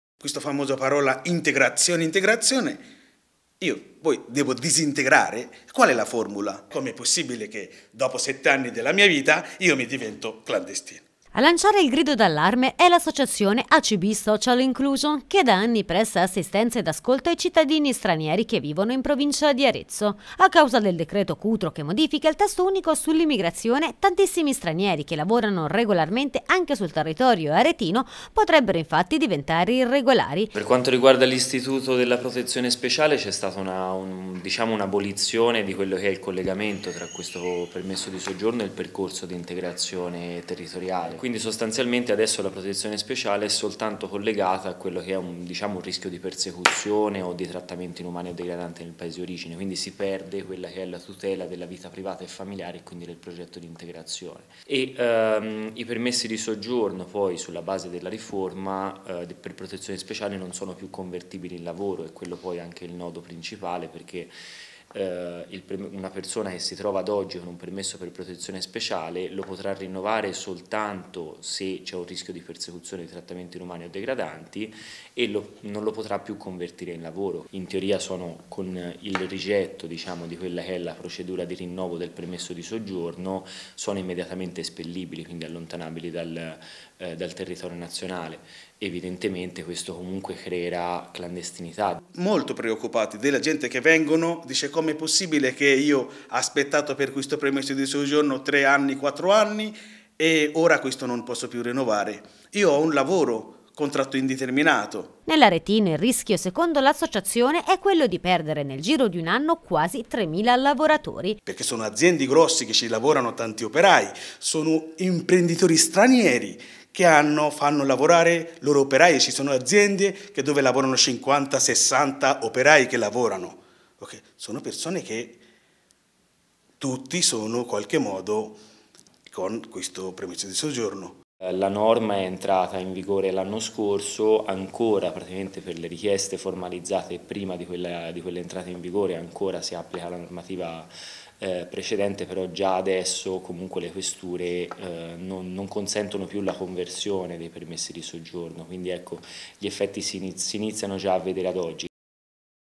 Decreto Cutro, Acb Social Inclusion: “a rischio 3mila lavoratori ad Arezzo”
A lanciare il grido d'allarme è l'associazione Acb Social Inclusion, che da anni presta assistenza e ascolto ai cittadini stranieri che vivono nella provincia di Arezzo.